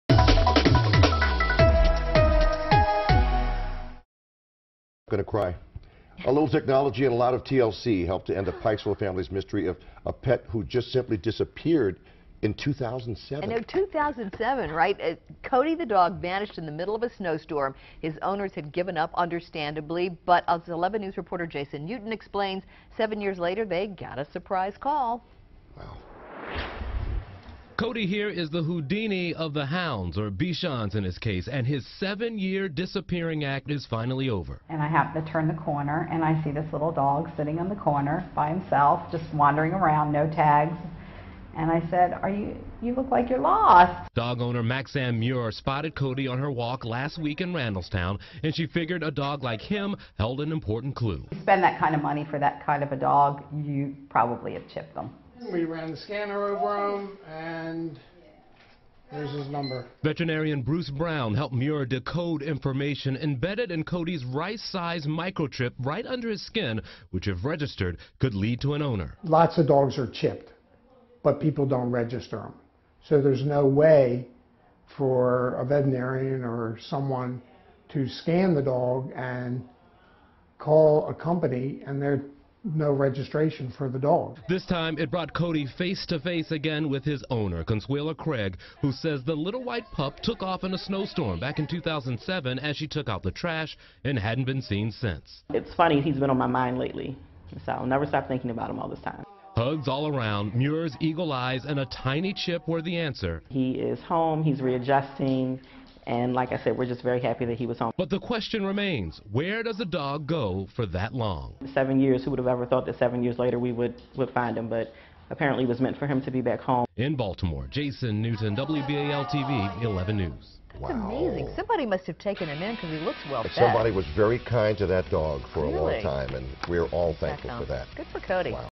Missing dog shows up 7 years later
Cody the dog vanished in the middle of a snow storm. His owners had given up, understandably, but seven years later they got a surprise call.